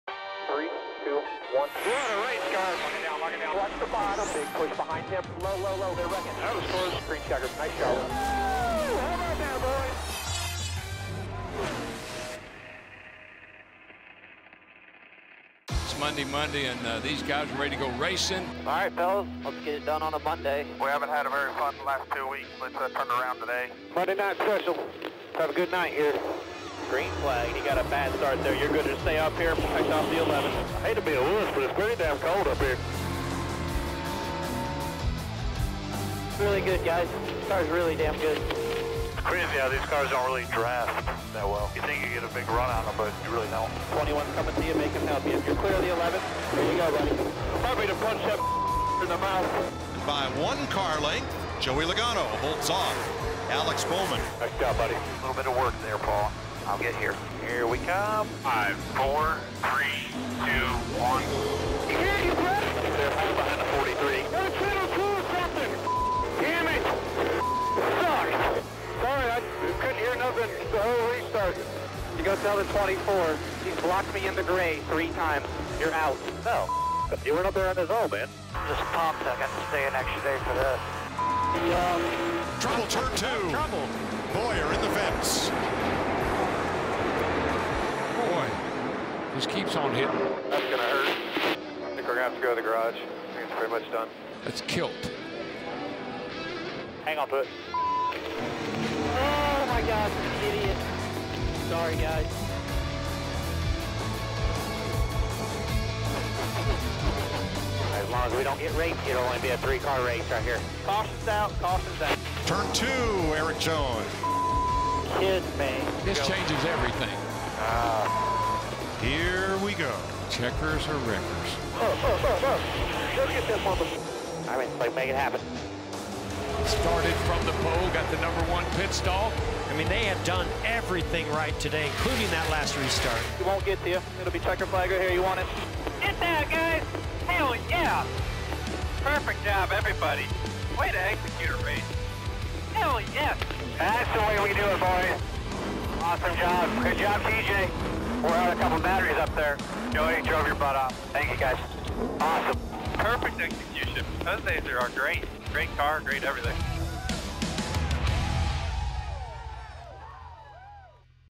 Scanner: 'Remind me to punch that (expletive) in the mouth'
Go inside the helmets of the Monster Energy NASCAR Cup Series drivers during the FireKeepers Casino 400 at Michigan.

Play every weekend!